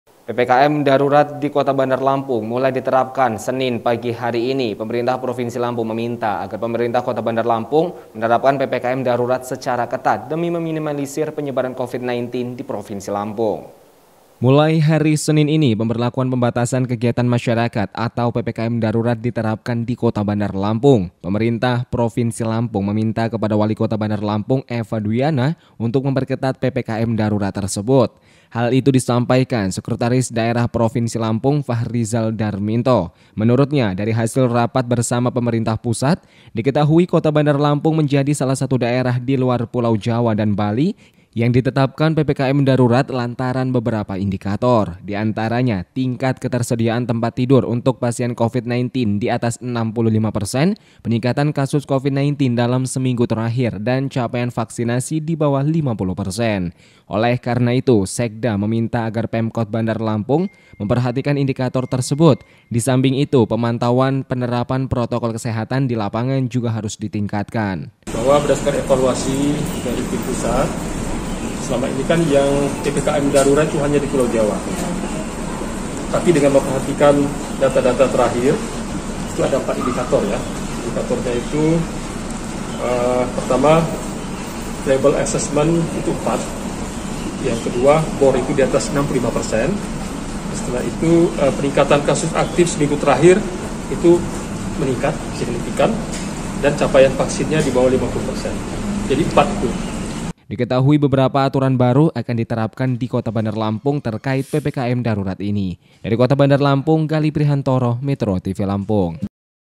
PPKM Darurat di Bandar Lampung Diberlakukan Secara Ketat
PPKM darurat di Kota Bandar Lampung mulai senin depan diterapkan. Pemerintah Provinsi Lampung meminta pemerintah Kota Bandar Lampung untuk menerapkan PPKM darurat secara ketat. Hal ini agar pengendalian kasus Covid-19 di Lampung bisa diminimalisir.